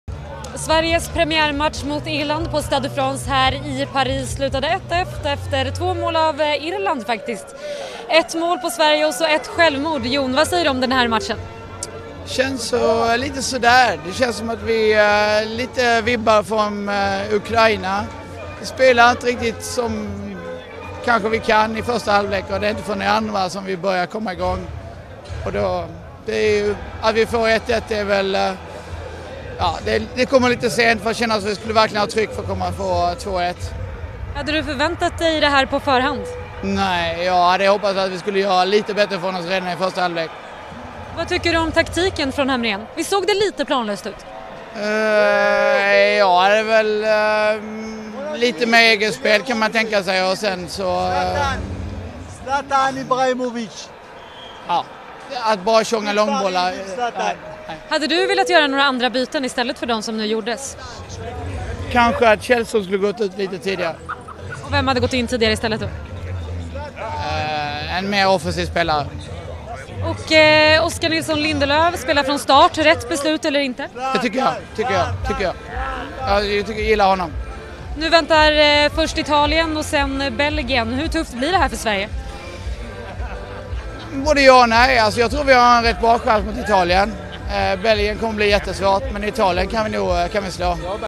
Sverigesupportern Jon: "Känns sådär just nu"
Sverige spelade 1-1 mot Irland i den första matchen för Sverige i EM i Paris.